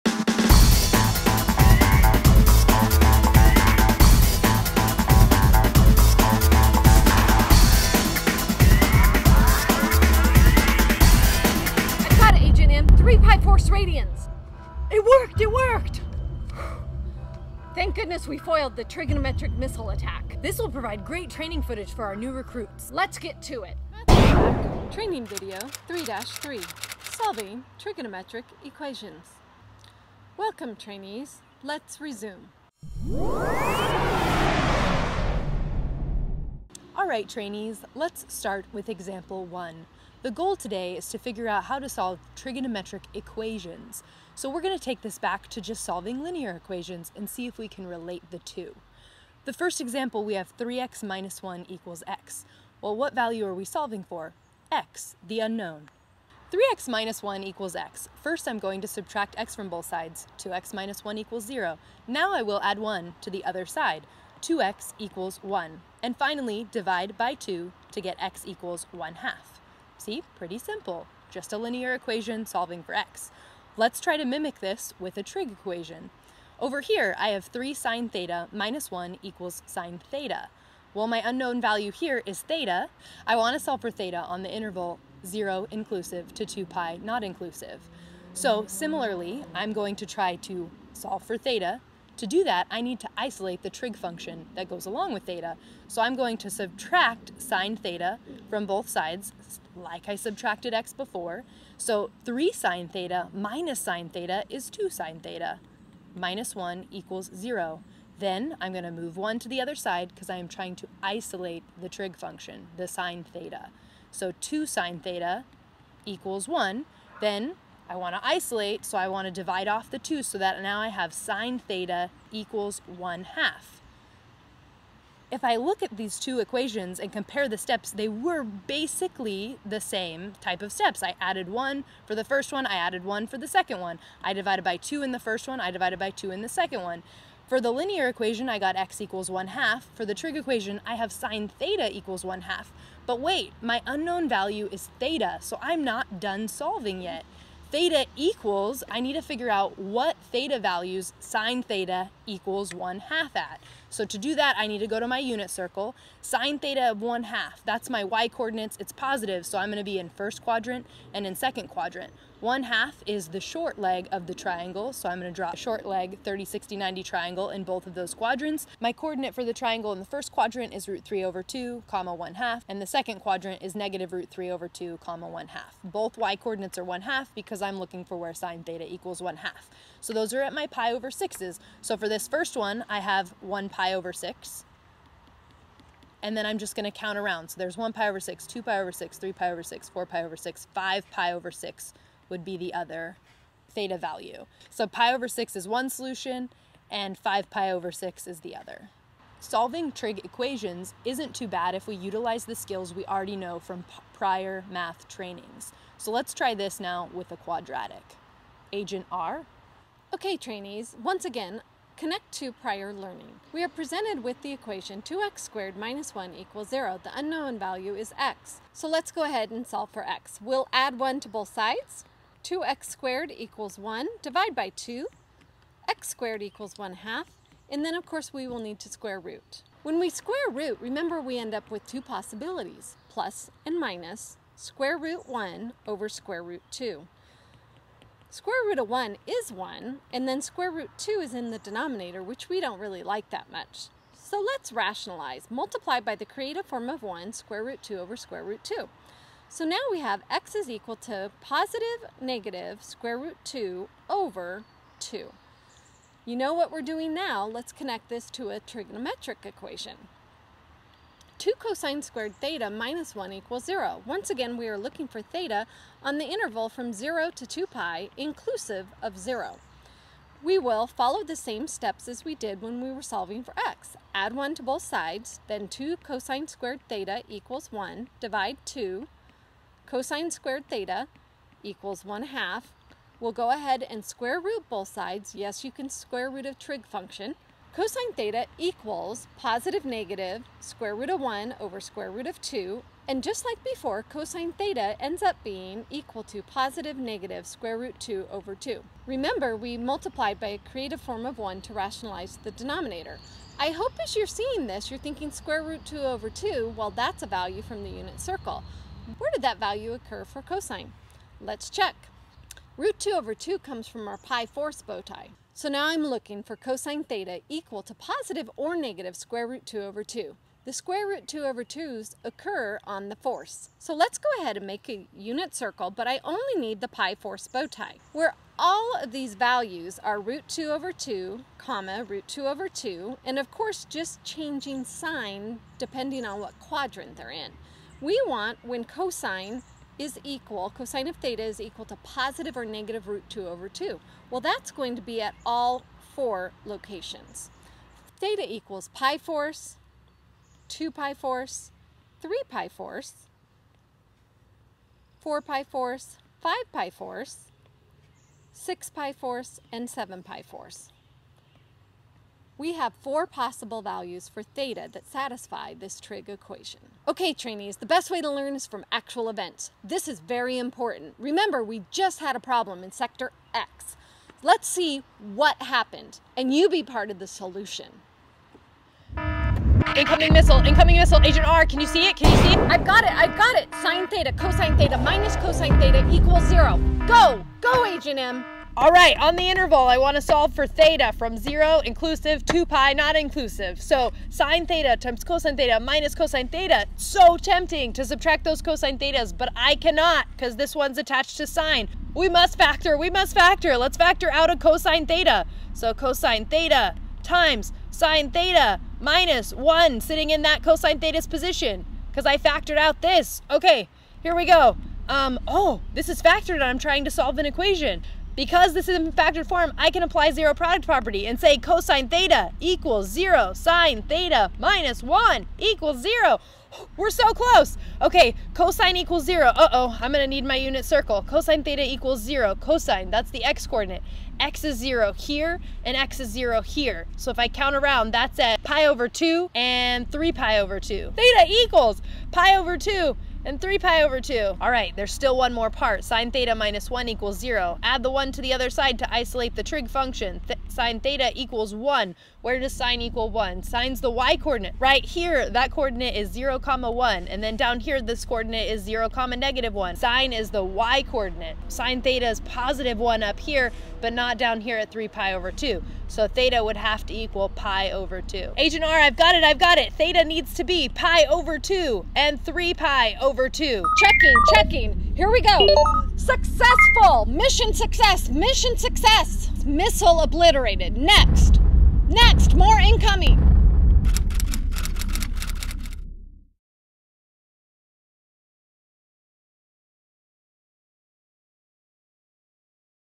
MIB Trig Training 3-3 Video 1
Video 1 of 2 - Math in Black Agents M and R solve trig equations to stop trigonometric missiles from infiltrating headquarters.
Learn how to solve trigonometric equations by isolating trig functions, factoring the GCf and factoring a quadratic.

Want to learn some more background knowledge about the Unit Circle and Sine, Cosine, Tangent check out this playlist